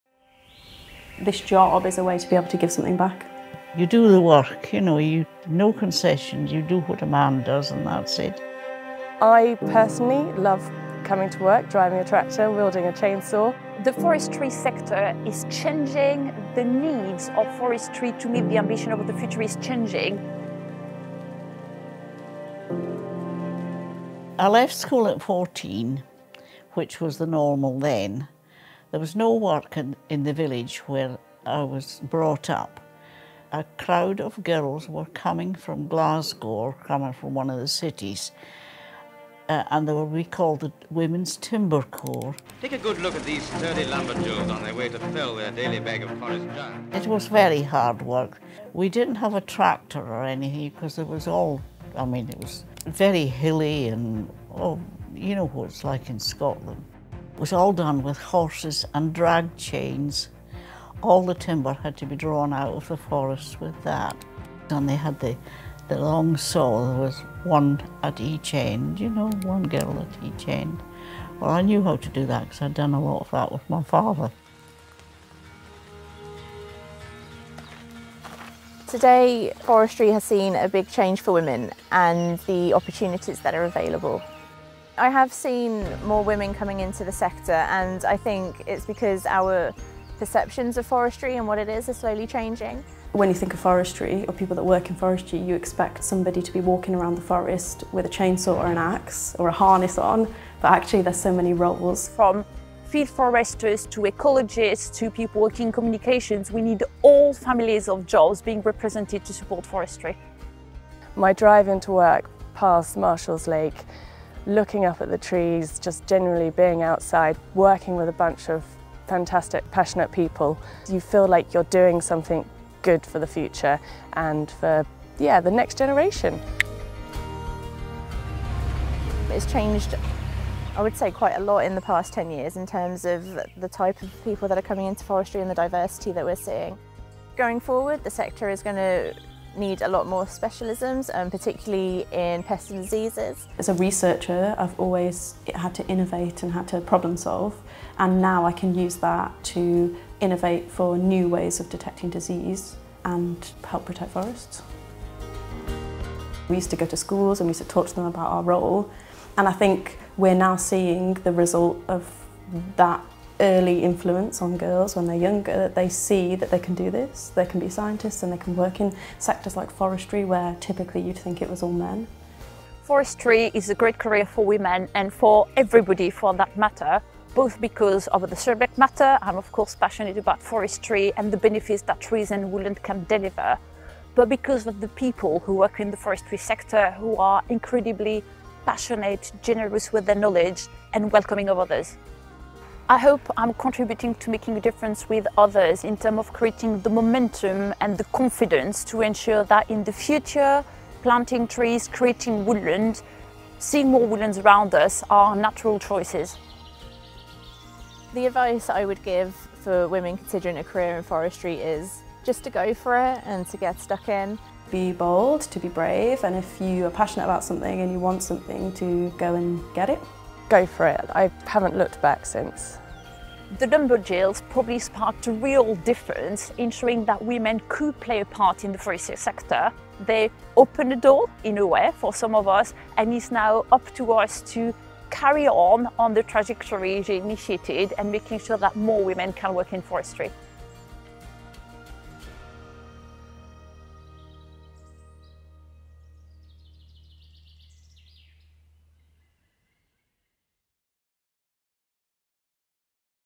The role of women in forestry.
As a new exhibition is set to open in Grizedale Forest we look at the roles of women in forestry, from the Lumberjills of the Second World War to the women working across the forestry sector today.

Find out more about Women in Forestry, the Lumberjills’ Story exhibition

And you can discover the careers available today Just search Forestry Commission.